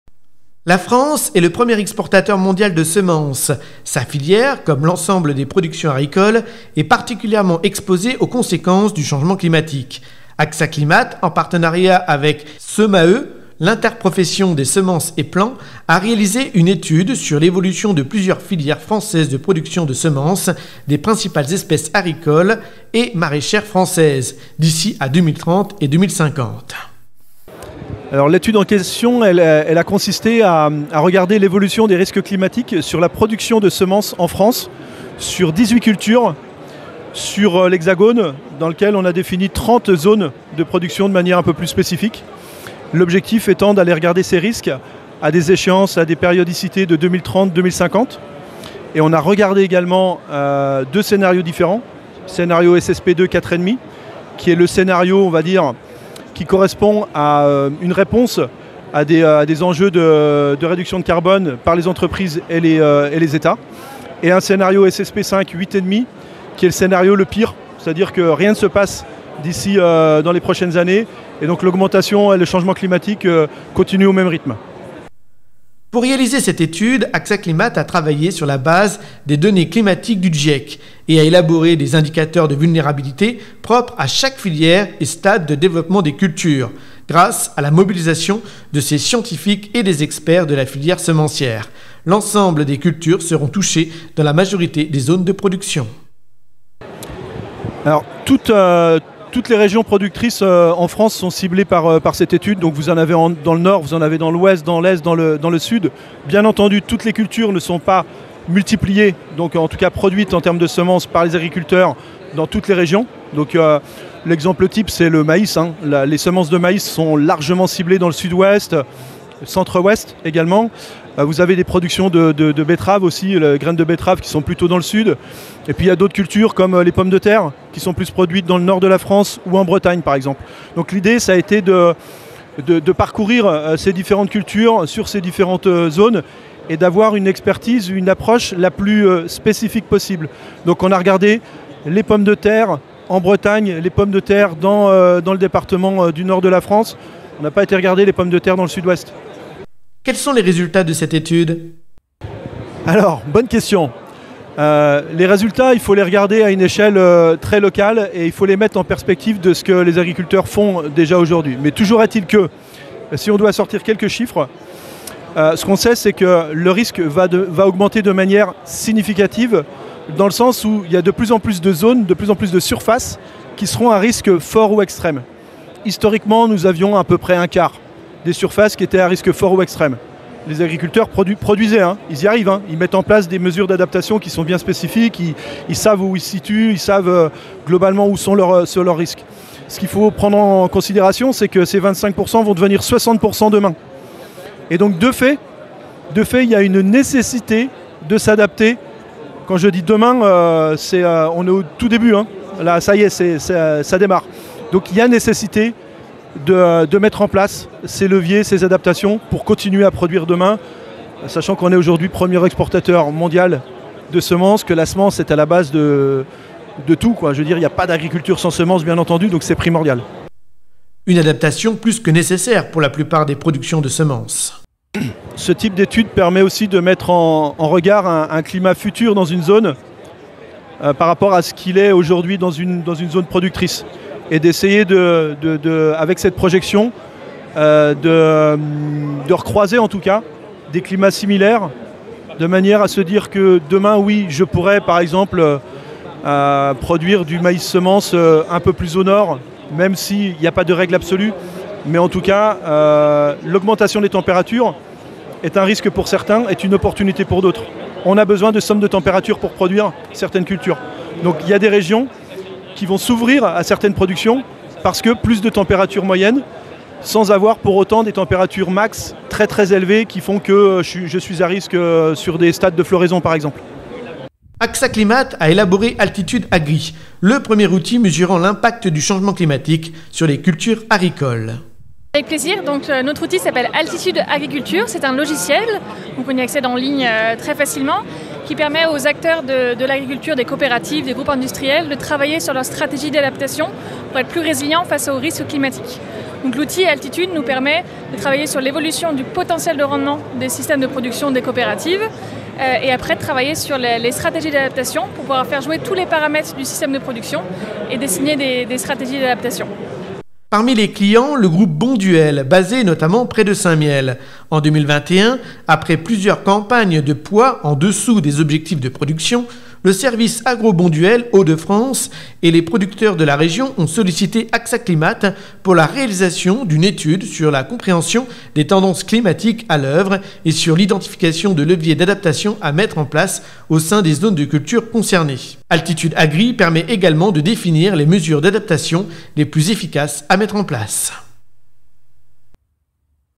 Climat: AXA Climate et SEMAE dévoilent une étude sur les semences de légumes et de céréales.
AXA Climate a dévoilé, en partenariat avec SEMAE, les résultats d’une étude inédite mesurant l’impact du changement climatique sur les semences de légumes et de céréales. Elle a présenté également Altitude Agriculture, le 1er outil de quantification du risque climatique sur les cultures agricoles.